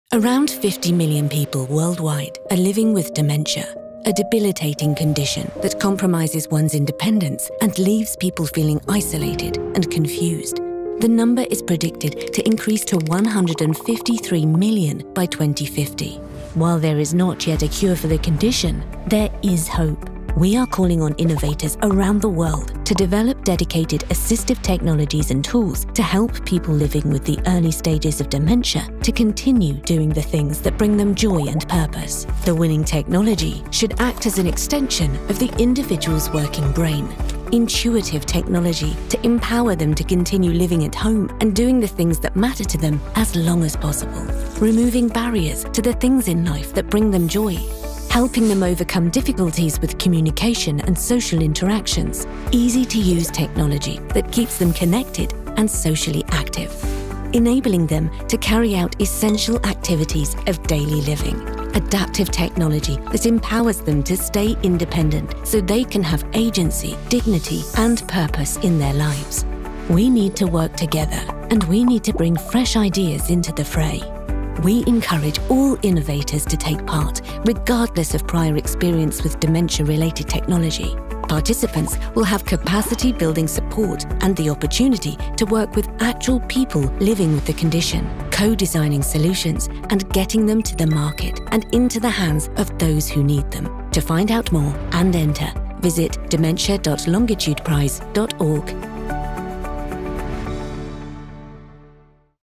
Longitude Prize on Dementia: Helping people with dementia retain their independence
The Longitude Prize on Dementia is a multi-million pound prize that will drive the creation of personalised, technology-based tools that are co-created with people living with the early stages of dementia, helping them live independently for longer and continue to do the things that bring them joy and purpose.

While there is not yet a cure for dementia, remaining independent in your own home and avoiding hospital admissions is one of the best ways to slow the progression of the disease. The winning solution will use the latest advances in technology, artificial intelligence (AI) and machine learning in combination with user data and testing to provide personalised support for people living with dementia. Find out more at dementia.longitudeprize.org